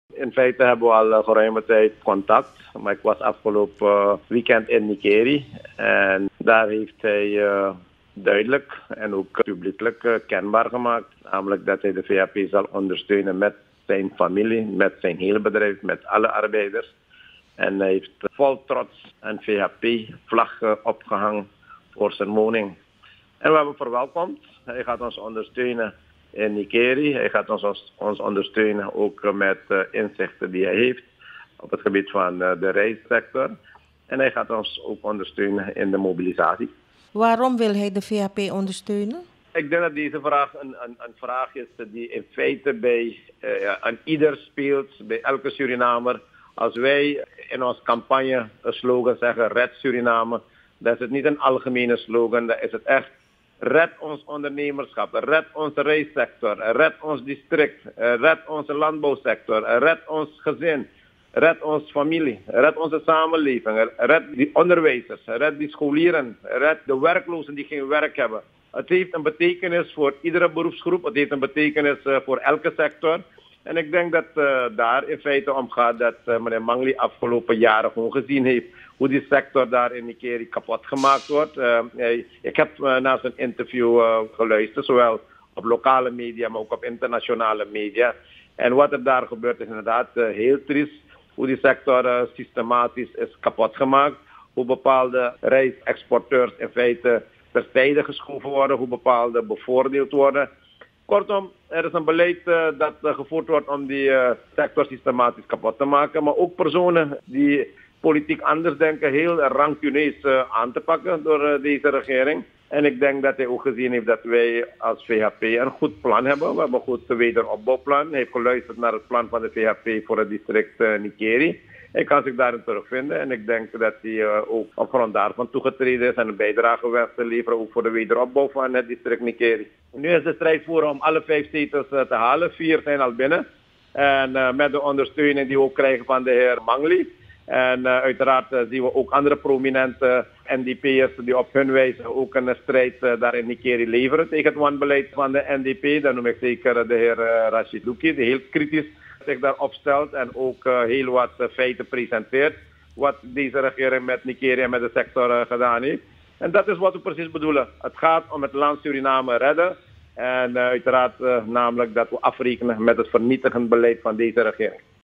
Bekende rijstondernemer Manglie ondersteunt de VHP - ABC Online Nieuws
ABC Online Nieuws - 12 mei 2020

Bekende rijstondernemer Manglie ondersteunt de VHP

De bekende rijstondernemer Imro Manglie uit Nickerie ondersteunt de VHP. Dit bevestigt VHP-voorzitter Chan Santokhi.

Manglie is ontevreden over het beleid van de NDP. Hij kan zich terugvinden in de plannen die de VHP gepresenteerd heeft.
Volgens Santokhi zal Manglie met zijn bedrijf en zijn familie, de VHP volop steunen in de campagne.